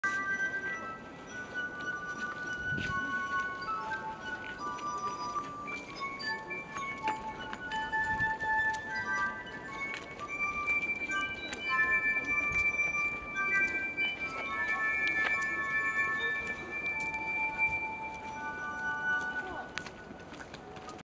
Performer on Charles Bridge
Playing music on glasses filled with water